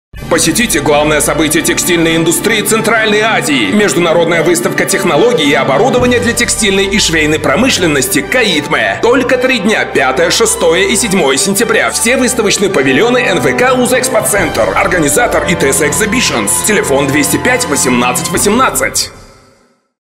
CAITME 2018
Запланируйте свой визит 5-7 сентября 2018 года на самую большую выставку технологий для текстильной и швейной промышленности в странах Центральной Азии - CAITME 2018!